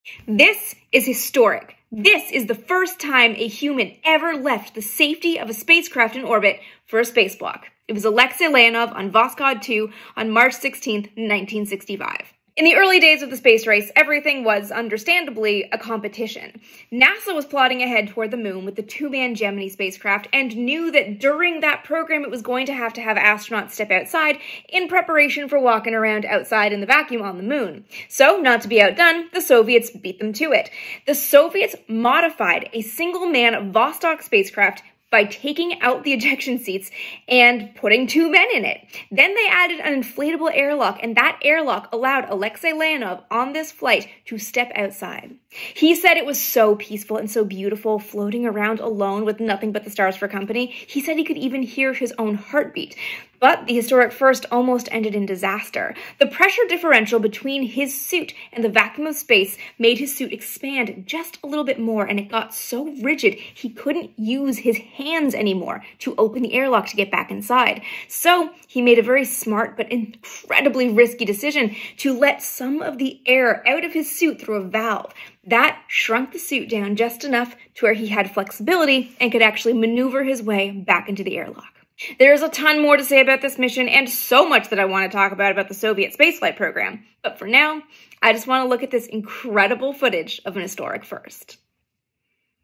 The First EVER Spacewalk
Another worthwhile part of this mission to mention: the spacecraft landed off target forcing Leonov and Pavel Belyayev to spend the night in the forest with nothing but their spacecraft for warmth. Recovery crews eventually reached them on cross country skis! Their second night in the forest they had a fire and company, which was a definite upgrade!